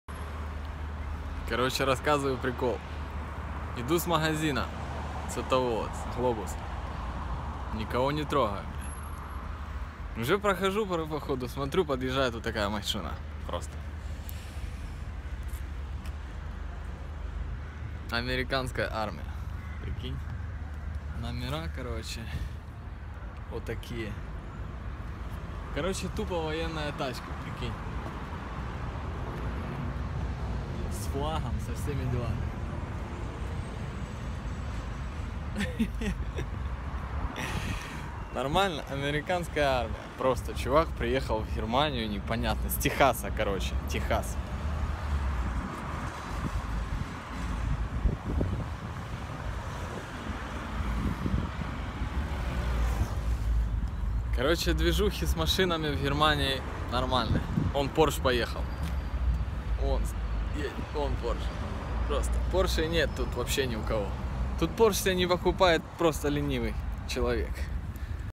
МАШИНА АРМИИ США В ГЕРМАНИИ
На парковке увидел автомобиль армии США с номерами Техаса.